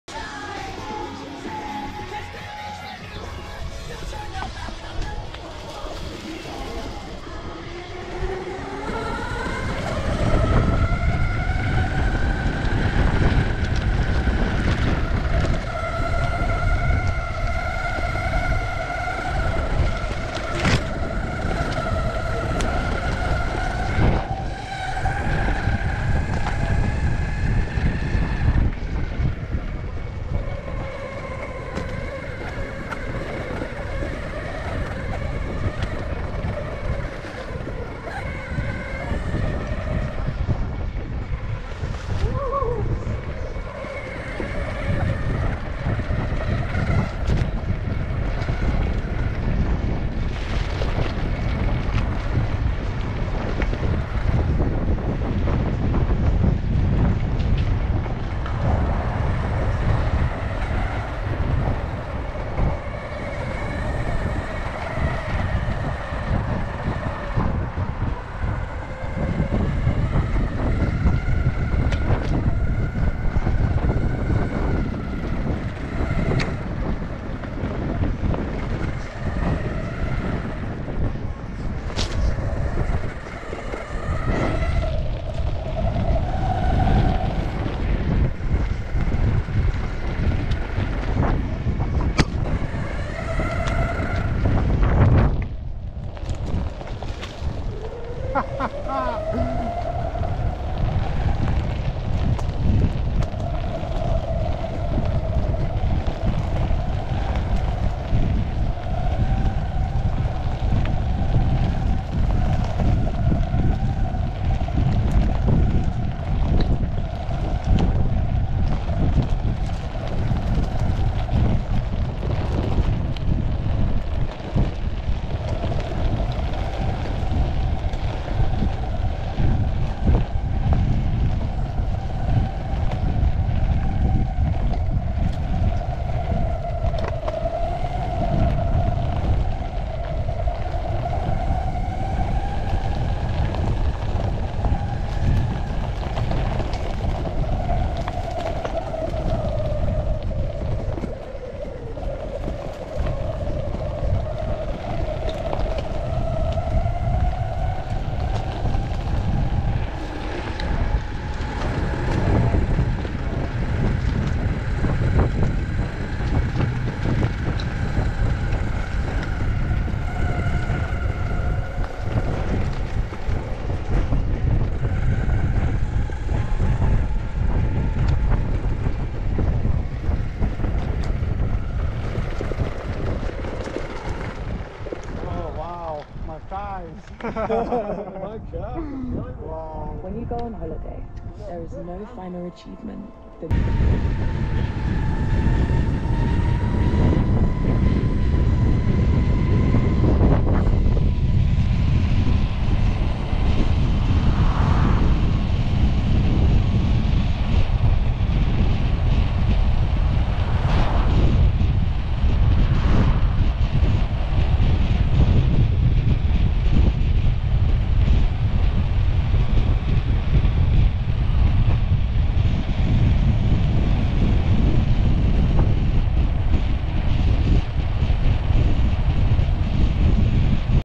Again trying to catch up with Steve and his Evolve Hadean electric skateboard
Me with my frozen to death Eovan GTS Carbon Pro, trying to catch Steve, an ex UK champion and his Evolve Hadean Bamboo. Such a great ride, luckily dry this time 🤙⚡🛹 ESK8 all the way